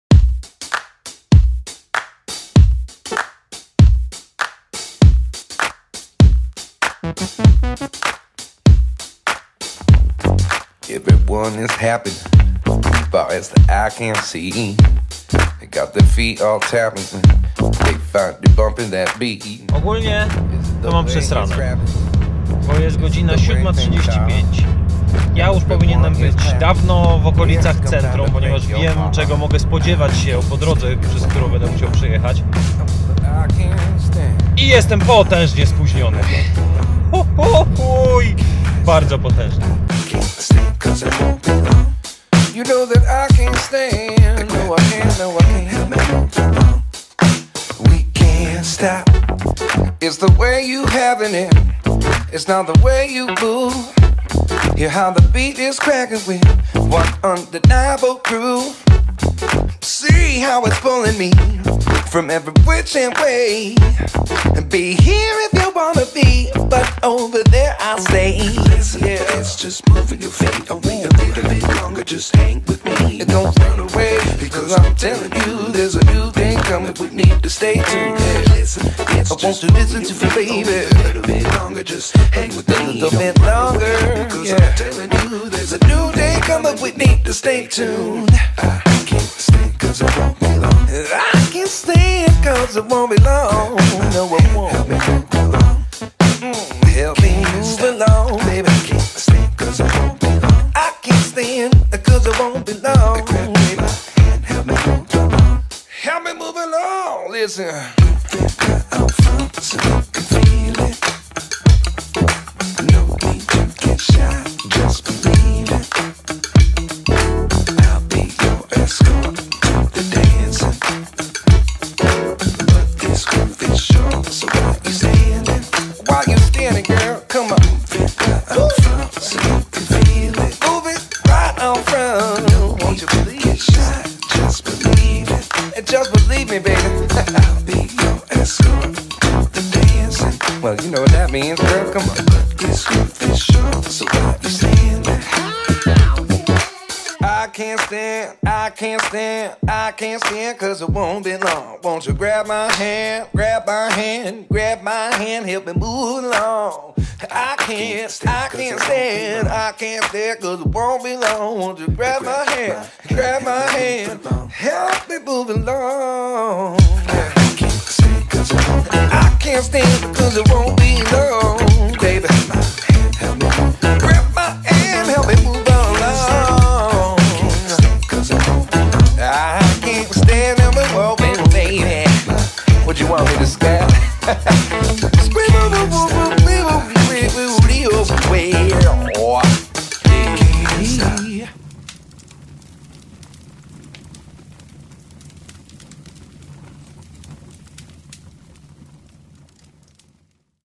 Zwyczajny dzień
I was always thinking: What my life would be like if it had a soundtrack? So from now on I'll post some videos showing that :)
#2  - Day in a life